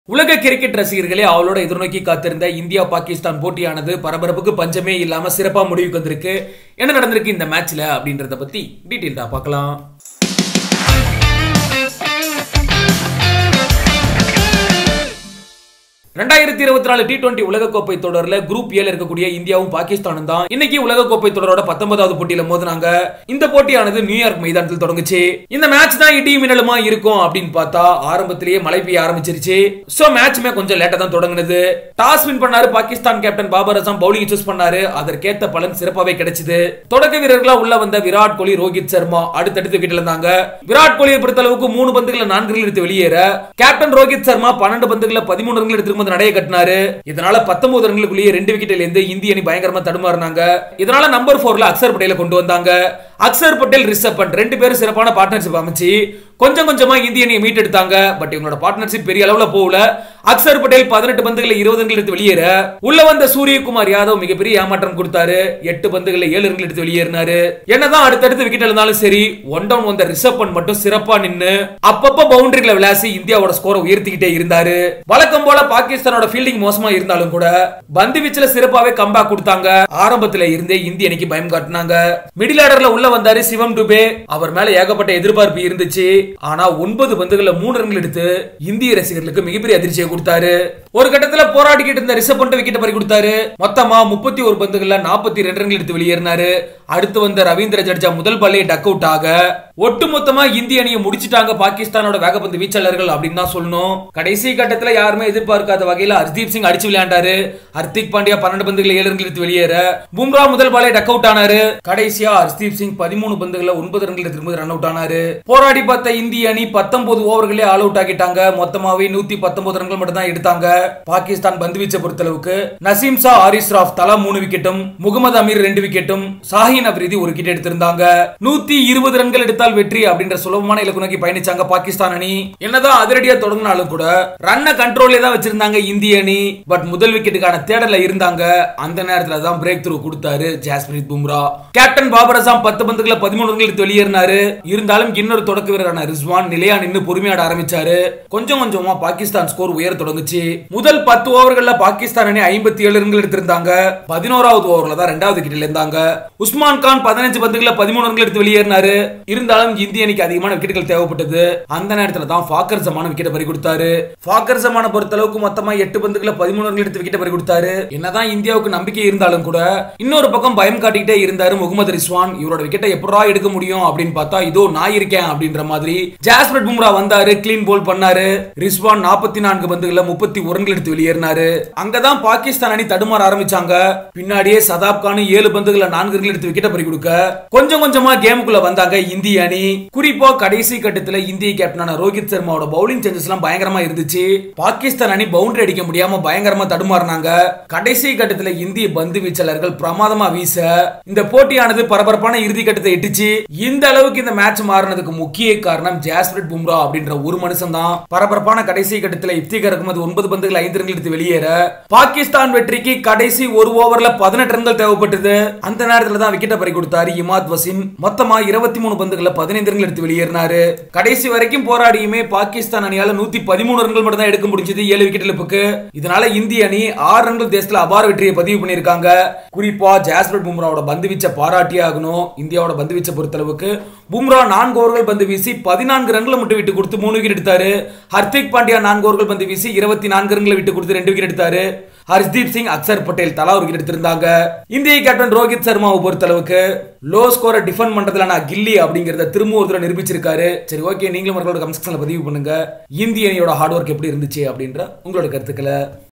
IND vs PAK ஆட்டத்தில் 6 ரன்கள் வித்தியாசத்தில் India அபார வெற்றி | Oneindia Howzat
T20 World Cup 2024 Update | India vs Pakistan Match Highlights | India won by 6 Runs